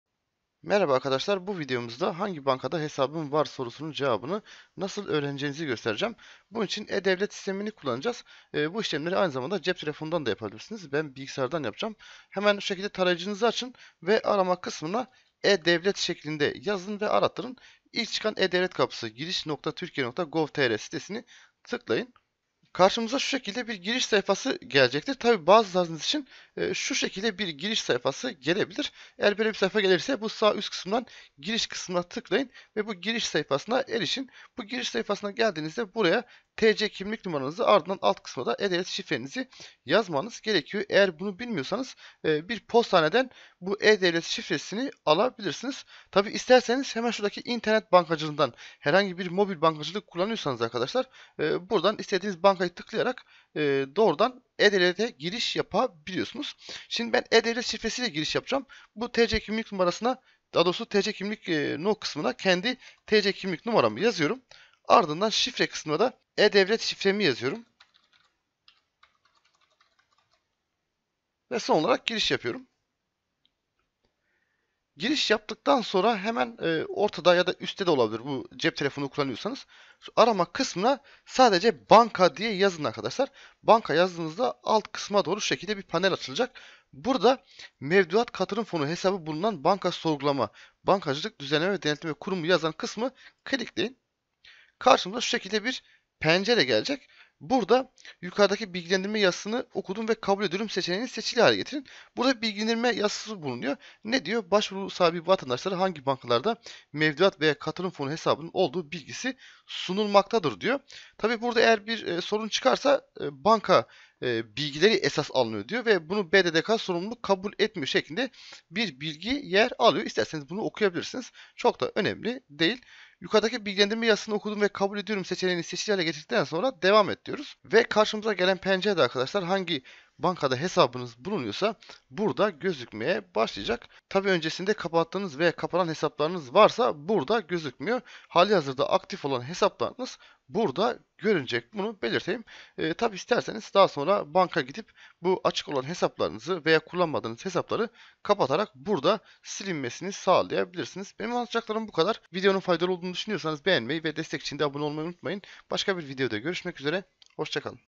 Hangi Bankada Hesabımın Olduğunu Nasıl Öğrenebilirim?
E-Devlet hizmetini kullanarak, bankalardan alınan veriler üzerinden hangi bankalarda hesabınızın olduğunu öğrenebilirsiniz.

Hangi bankada hesabınız olduğunu öğrenmenin en kolay ve hızlı yollarından biri de e- devlet web sitesini ziyaret etmektir. İnternet şubeleri aracılığı ile başka bir şifreye ihtiyaç duymadan, güvenli ve hızlı bir şekilde e-devlet Kapısı’na giriş yapabilirsiniz. Bunun için aşağıda vermiş olduğumuz resmi internet sitesini ziyaret etmeniz gerekmektedir.

Bu yöntem sayesinde ülkemizde adınıza hangi bankada hesap olduğunu, hesapların açık veya kapalı olup olmadığını, hesapların ne zaman açıldığını ve hangi şubelerde açıldığını, açık hesaplarınızın hareket dökümlerini görmeniz mümkündür.

Önemli Not: kişinin bu gibi durumlarla uğraşmaması için, herhangi bir sebepten açmış olduğu veya artık kullanmadığı hesabını anında kapatması gerekmektedir.